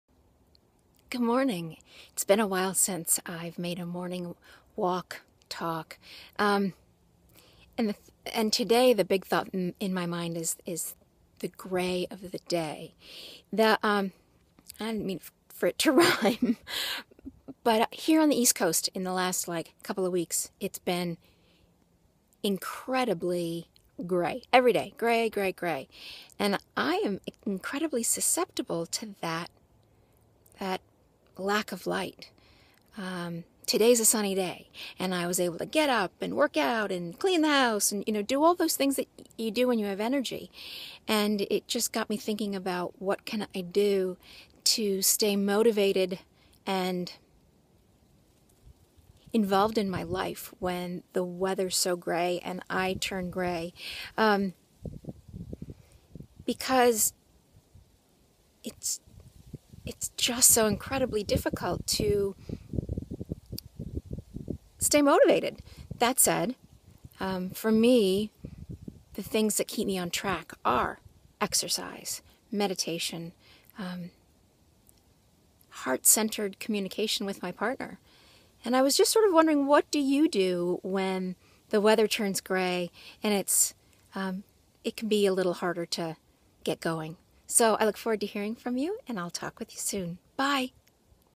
Sarah Hamilton Coaching Morning Walk #23
Good to see you all again!  How do you handle the grey days that we are having here on the east coast? I would love to know what you do?   Look forward to seeing you soon! Lots of Love! ~Sarah